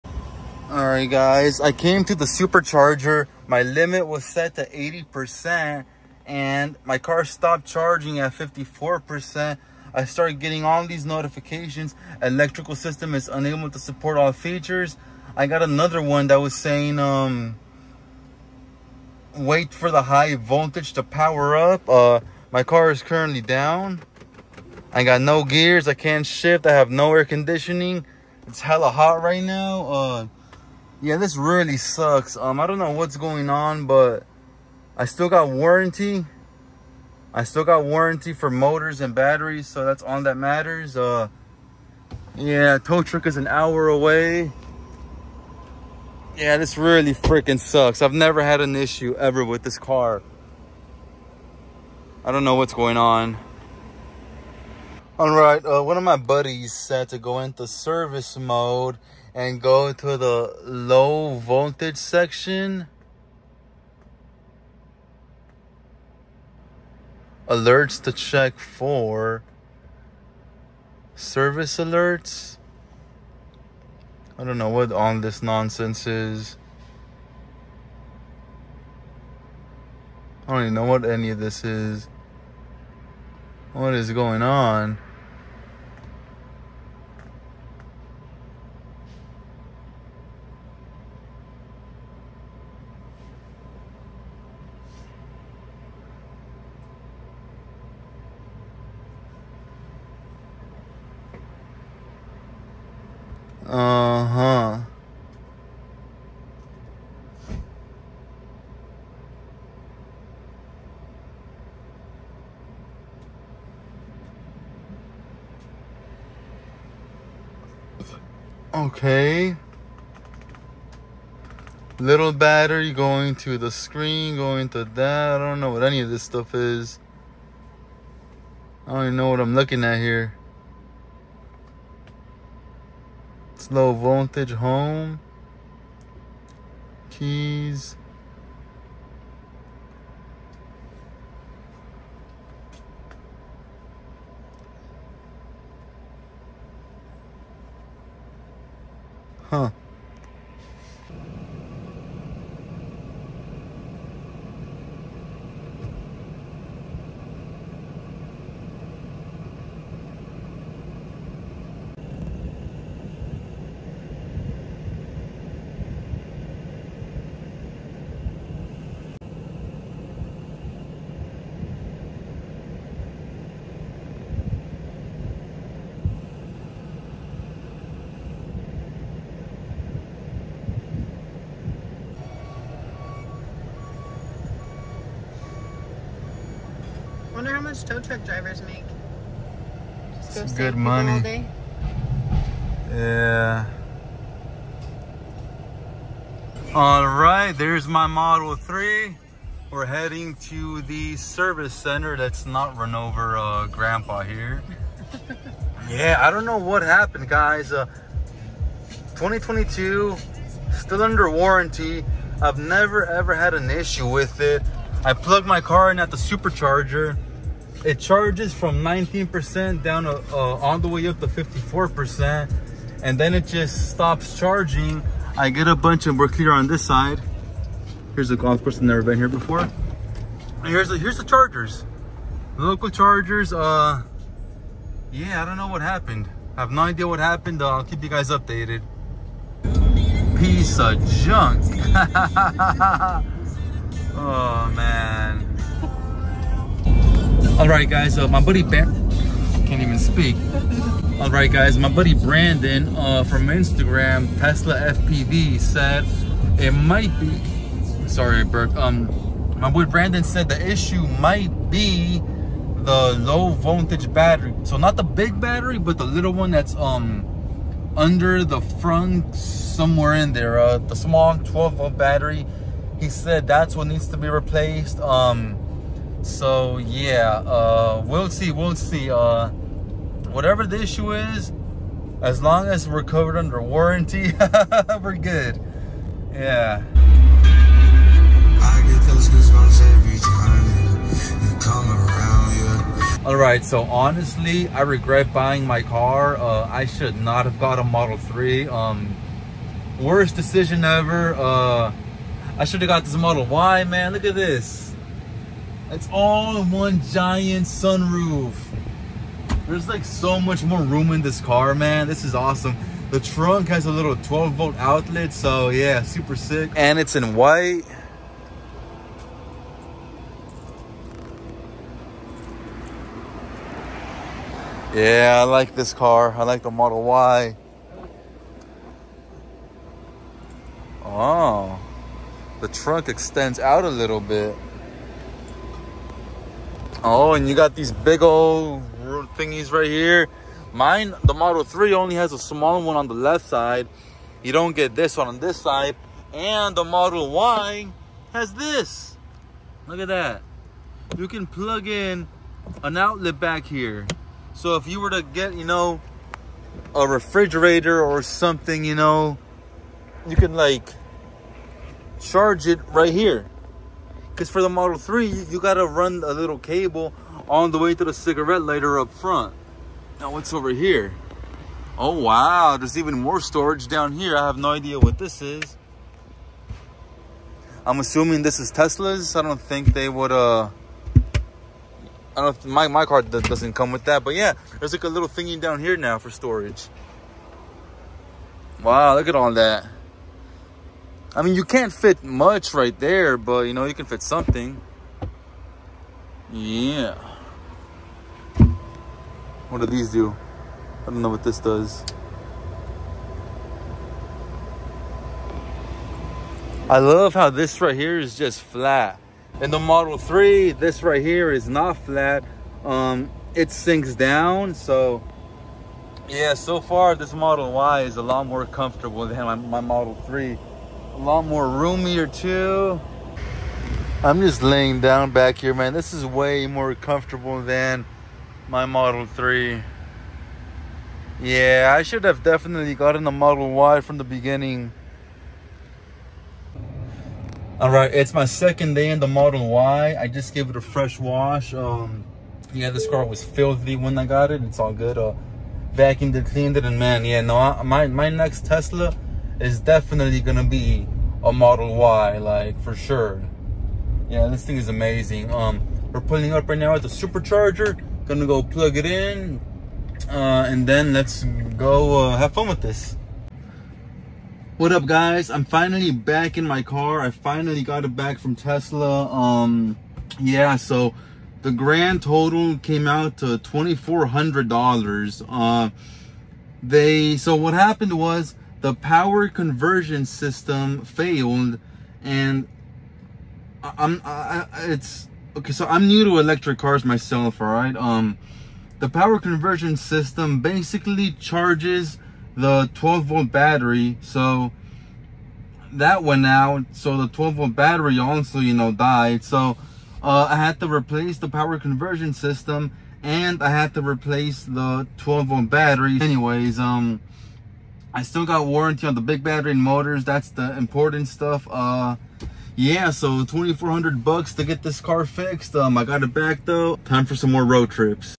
Tesla Model 3 Power Conversion System Failure!
At a little over 101,000 miles my Tesla suffered it's first ever failure.